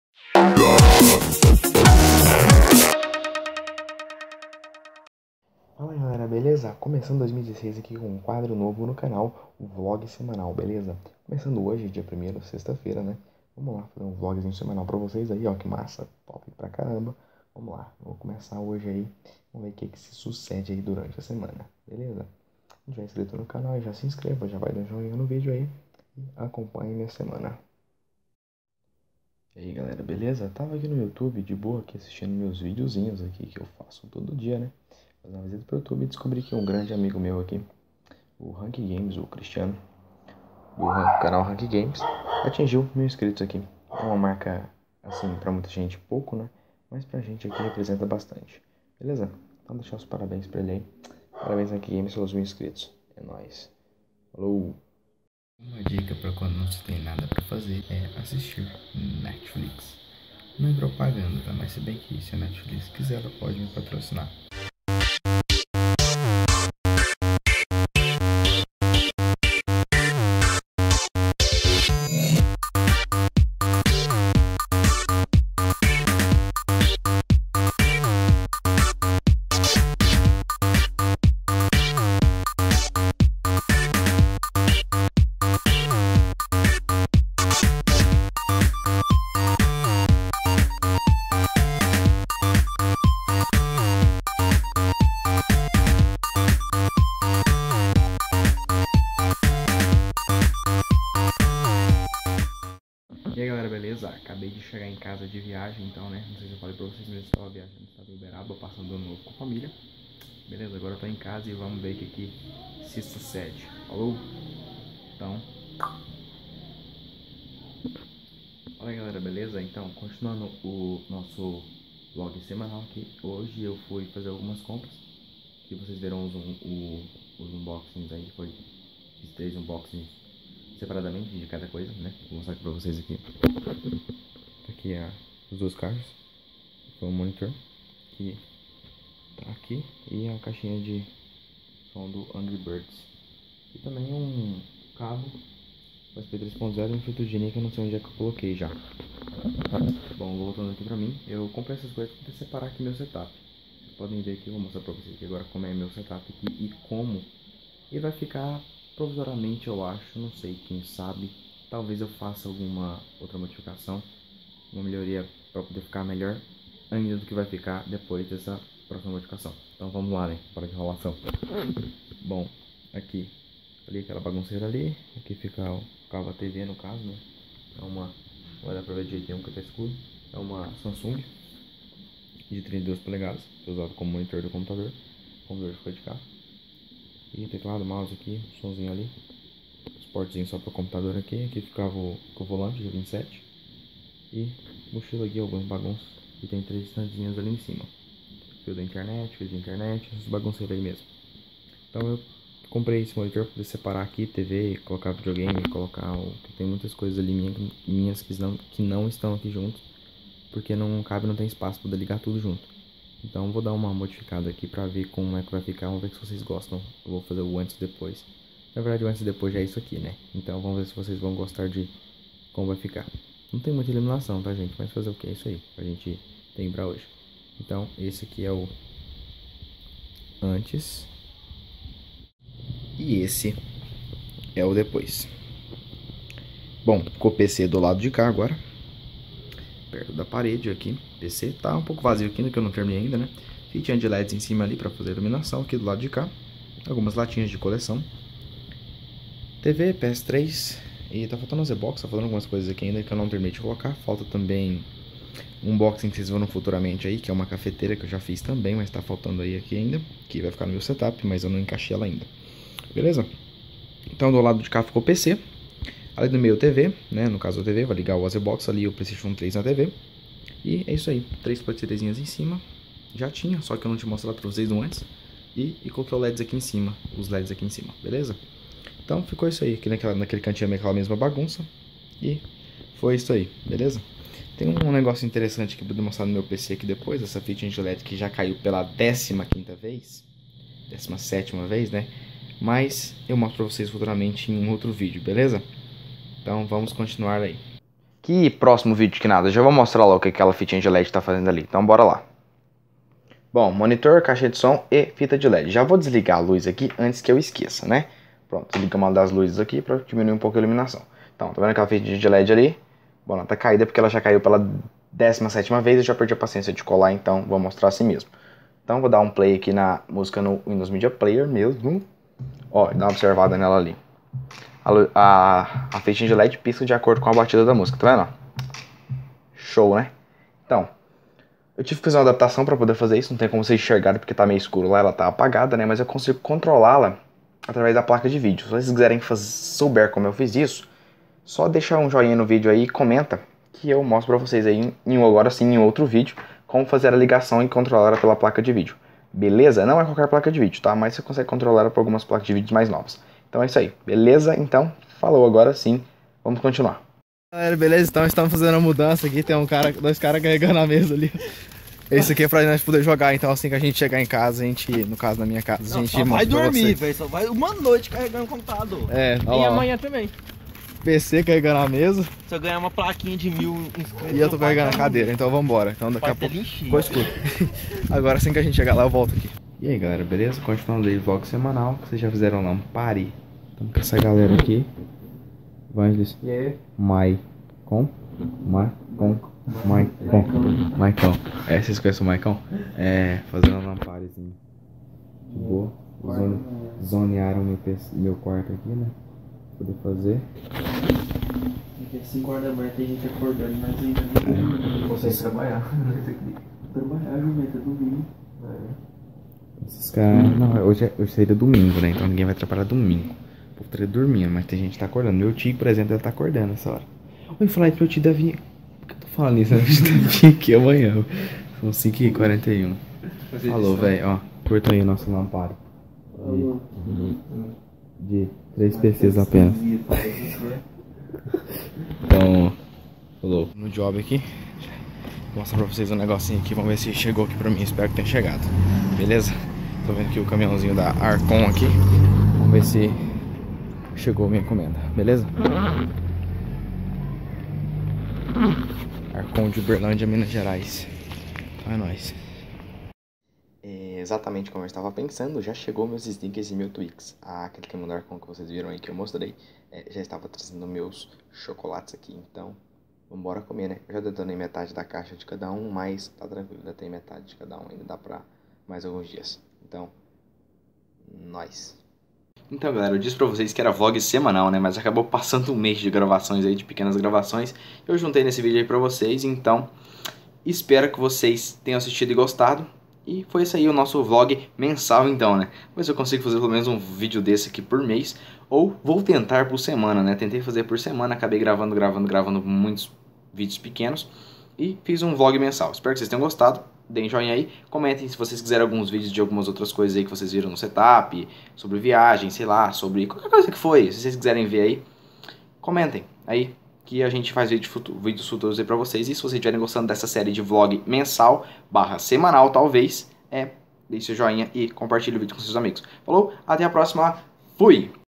Ano Novo, Viagem, Compras, Setup, LanParty e Encomendas - Monthly Vlog
Neste vlog que era para ser semanal e acabou sendo mensal falo sobre o uma viagem que fiz para passar o ano novo com minha família em Minas Gerais, faço algumas compras para modificar meu setup, junto com alguns amigos para fazer uma lanparty, recebo algumas encomendas e claro alguns extras que só assistindo o vídeo para saber espero que tenham gostado.

Use a Hashtag